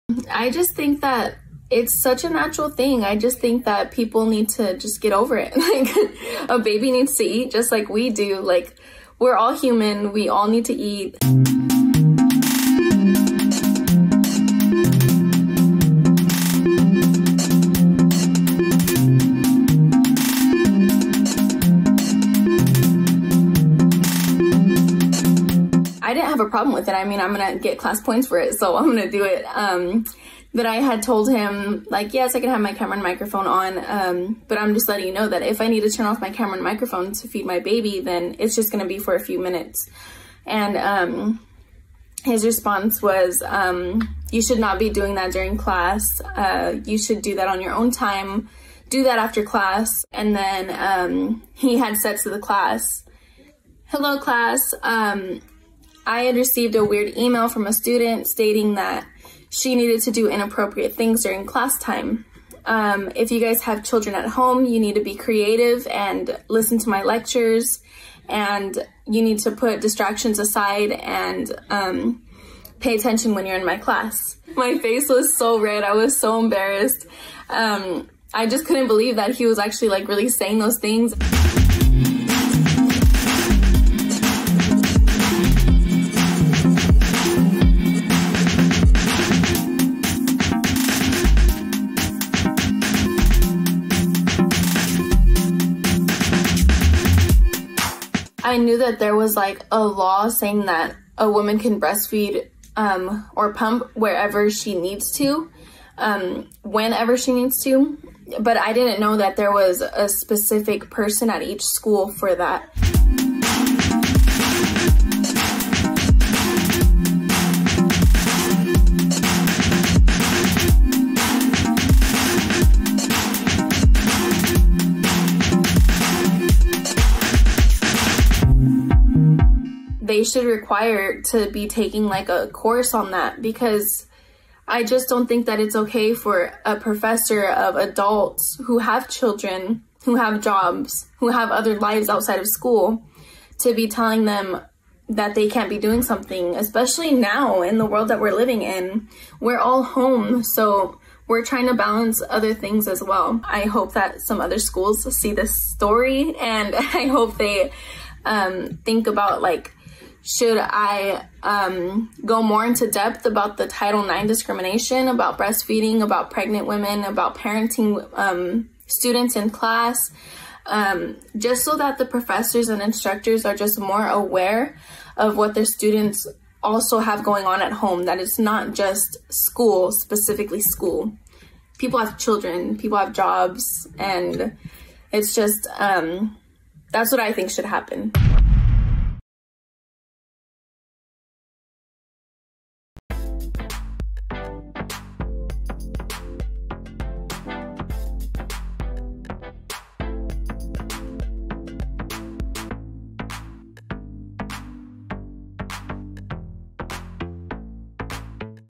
Class में स्तनपान कराने पर शर्मसार की गई छात्रा ने ये किया!
Class में बच्चे को दूध पिलाने पर शर्मसार की गई छात्रा ने teacher को यूं सिखाया सबक...